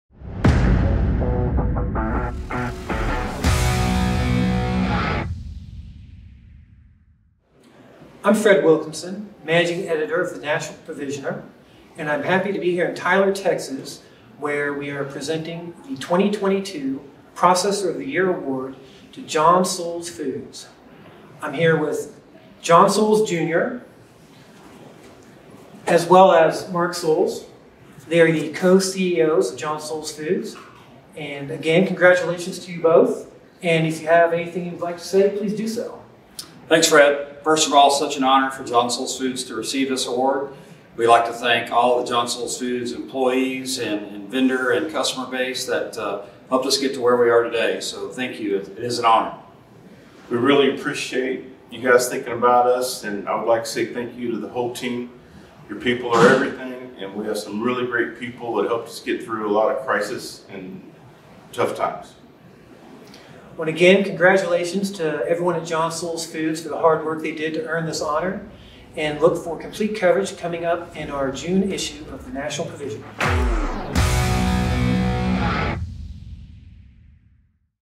The National Provisioner’s 2022 Processor of the Year: John Soules Foods
Powered by record sales, innovative branding, and a newly constructed state-of-the-art plant in Alabama, John Soules Foods has earned The National Provisioner’s 2022 Processor of the Year Award. Read the whole story in our June e-magazine.